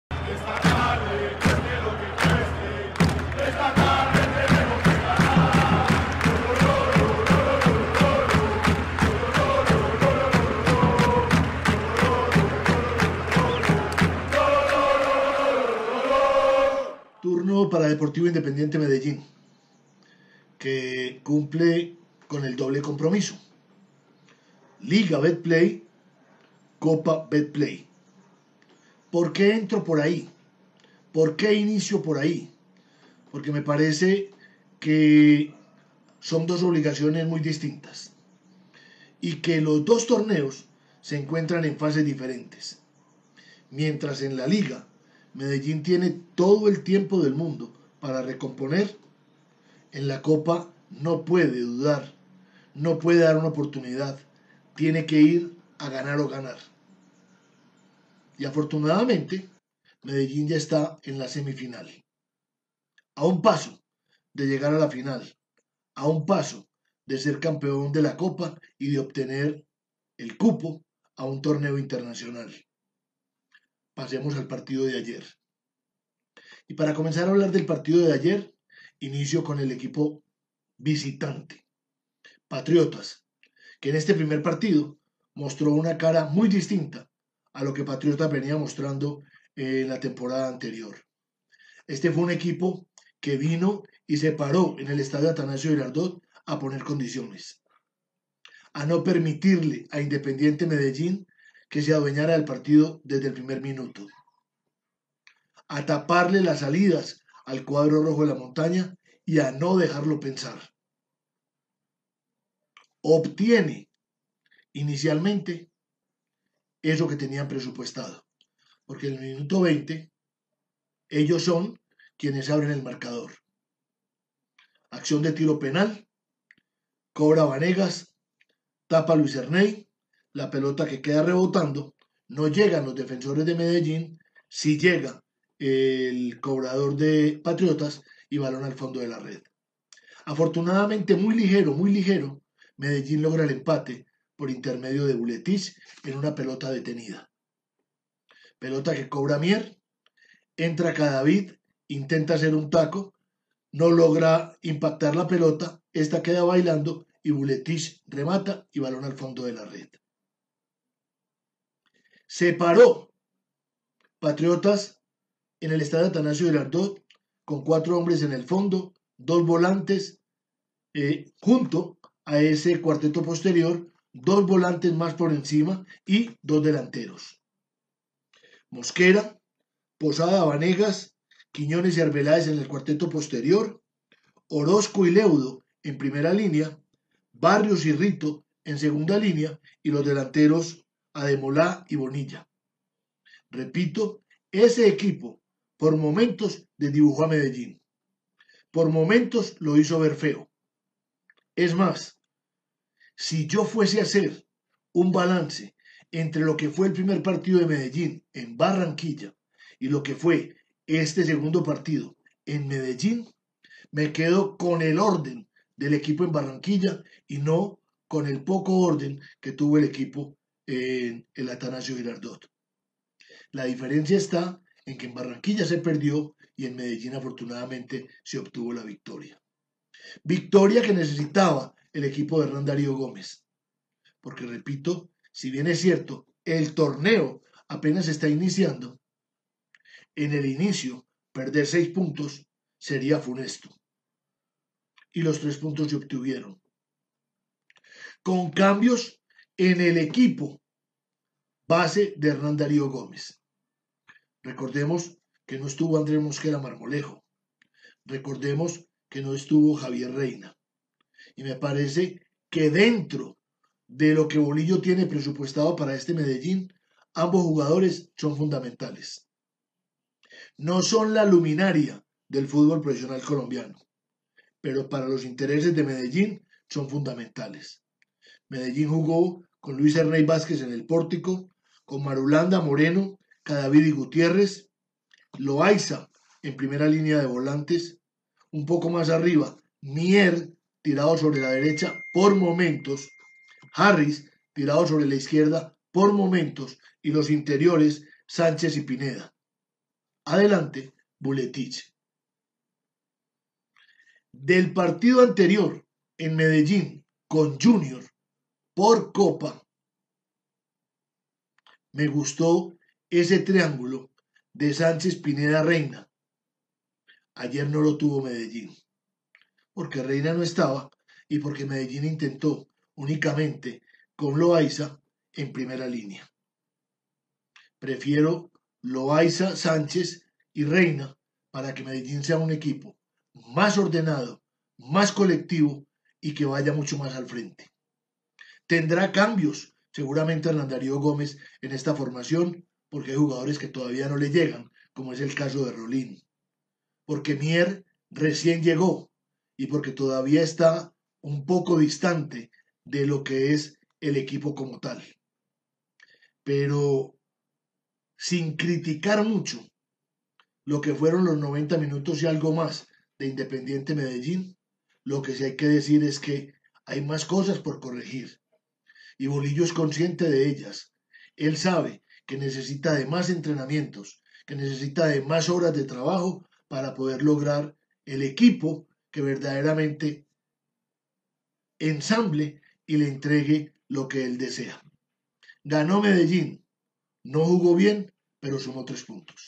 Cultura Fútbol /Análisis DIM 2 - Patriotas 1
Héctor Agudelo nos da su visión de lo que fue el triunfo del Deportivo Independiente Medellín ante Patriotas.

Ficha técnica:

Medellín: Luis Vásquez; Juan Guillermo Arboleda, Víctor Moreno, Andrés Cadavid, Germán Gutiérrez; David Loaiza, James Sánchez (Yulián Gómez m.90), Matías Mier; Jean Pineda (Yesid Díaz m.45), Robert Harrys (Leonardo Castro m.76) y Agustín Vuletich (Juan Díaz m.79).
Entrenador: Hernán Darío Gómez.

Patriotas: Carlos Mosquera, Oscar Vanegas, Jorge Posada (Jhon García m.68), Mairon Quiñones, Federico Arbeláez; Santiago Orozco, José Leudo, Edgardo Rito, Cristian Barrios; Arley Bonilla (Misael Martínez m.68) y Kevin Aladesanmi (Jesús Arrieta m.76).
Entrenador: Francisco Segovia.

Goles: 0-1 Oscar Vanegas m.19; 1-1 Agustín Vuletich m.23; 2-1 Agustín Vuletich m.70.
Amonestados: David Loaiza (DIM), José Leudo (PAT), Misael Martínez (PAT).
Estadio: Atanasio Girardot.
Árbitro: Nicolás Gallo.